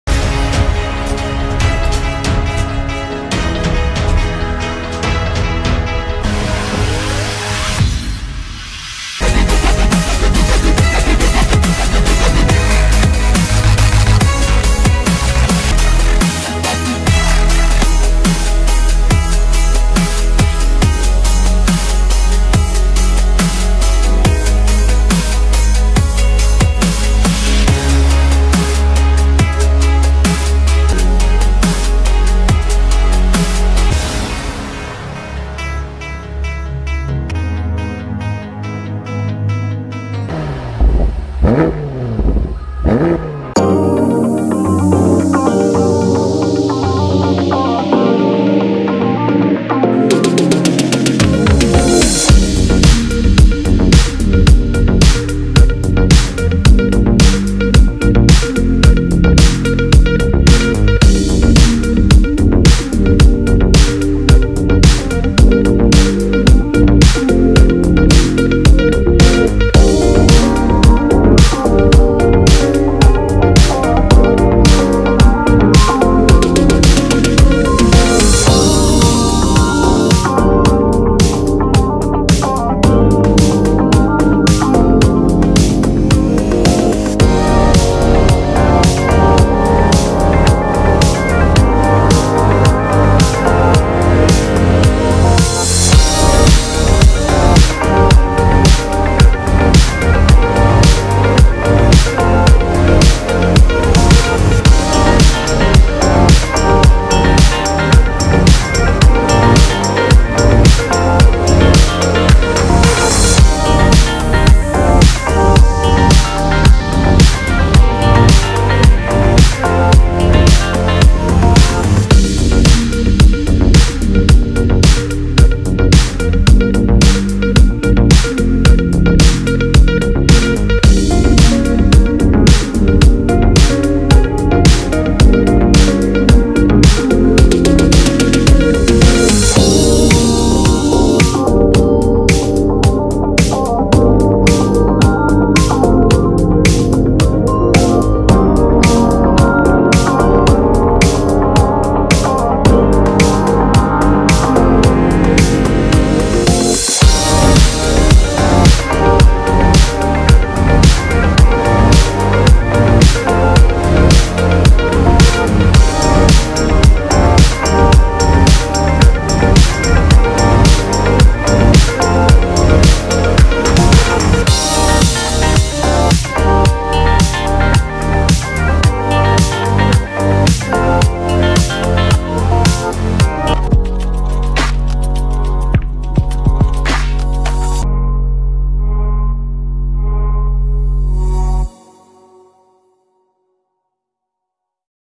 2021 BMW X6 M - Sound, Interior and Exterior Details - / 2021 BMW X6 M
BMW X6 M by Ramon Performance (2021) - Sound, Interior and Exterior

Perfomance:
0-100 (km/h): 3.9 s